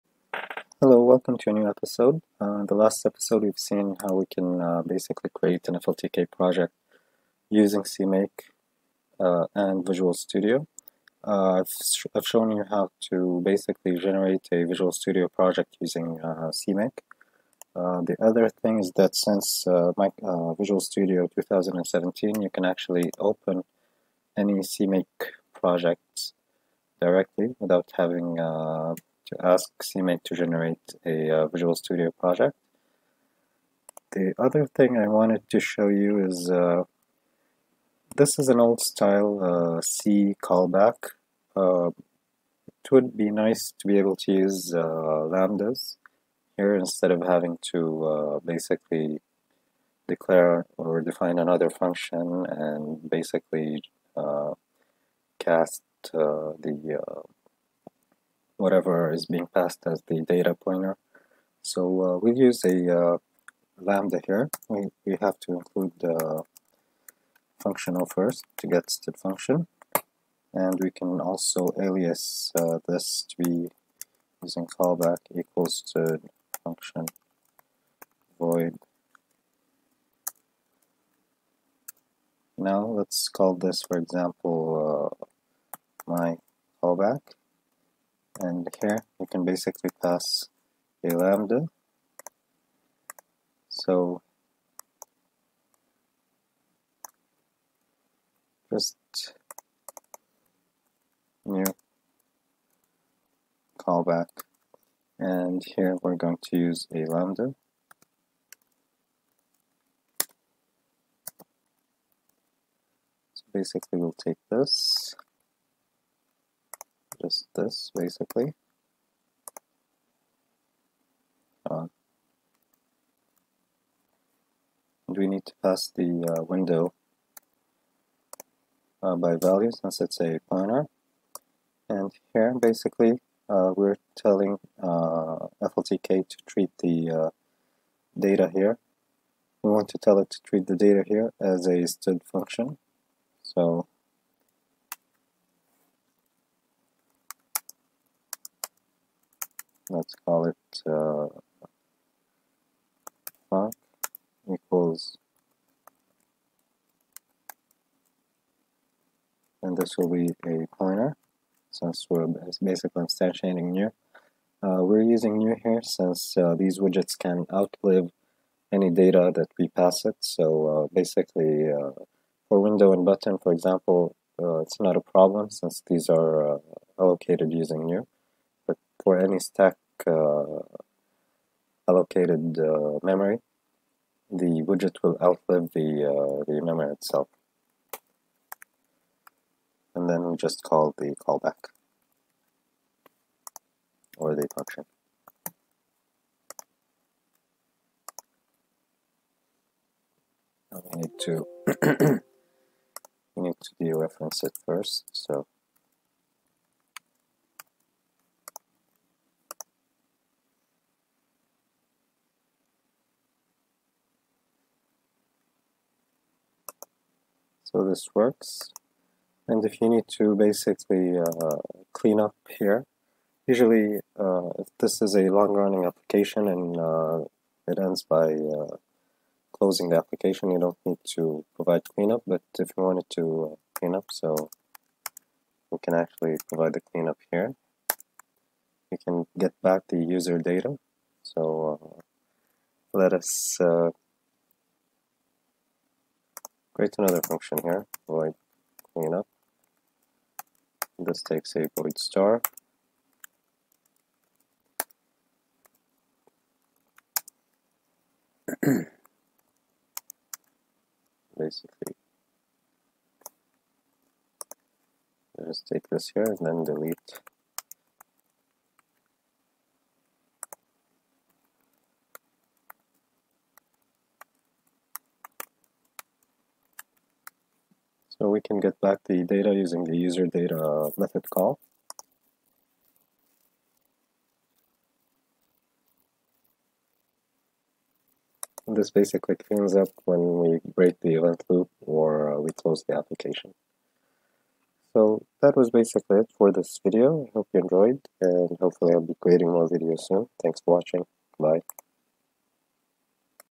Using modern C++ lambdas with FLTK
This video shows how to use modern C++ lambdas with FLTK. For a cleaner api, you can subclass a widget and have the callback function take a std::function, store the std::function in the derived class and pass the pointer to the parent widget.